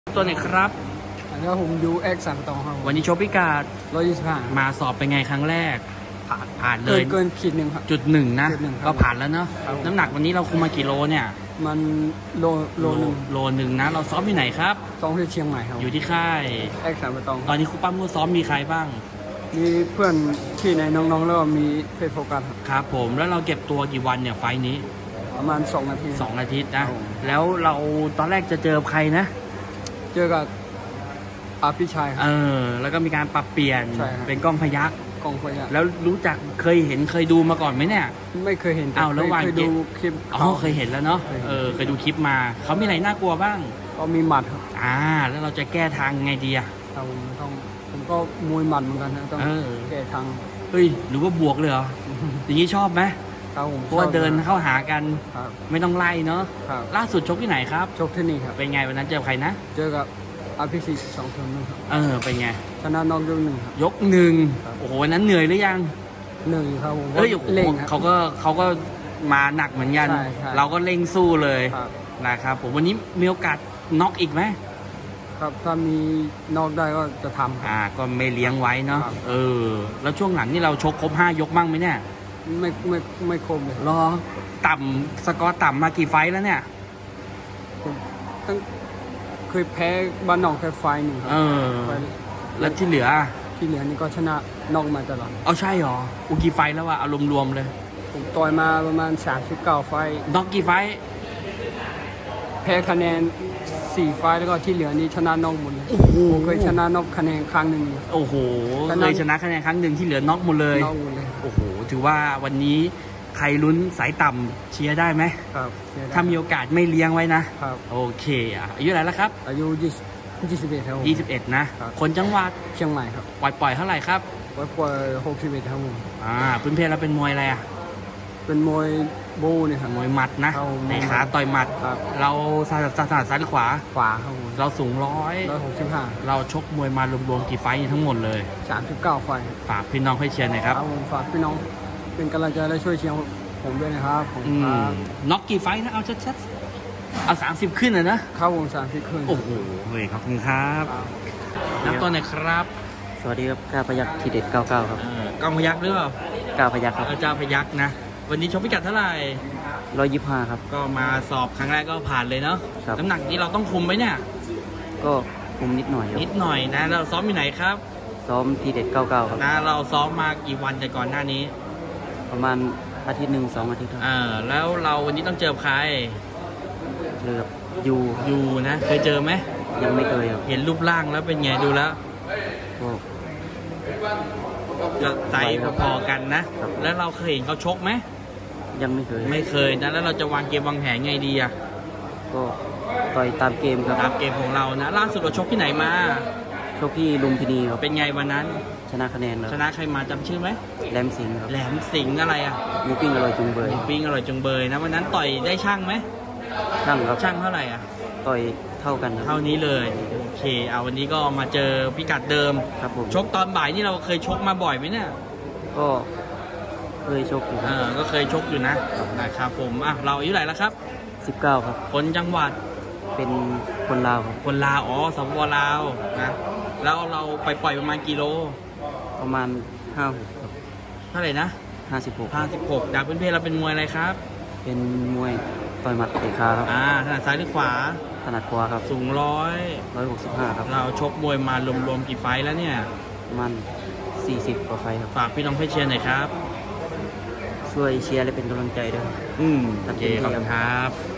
สัมภาษณ์คู่ที่3🔴 ยู แอ็ดสันป่าตอง พบ 🔵 กล้าพยัคฆ์ ทีเด็ด99
ศึก ๙ มวยดีวิถีไทย วันเสาร์ที่ 29 เมษายน 2566 เวทีมวย จิตรเมืองนนท์ เวลา 15:00 น.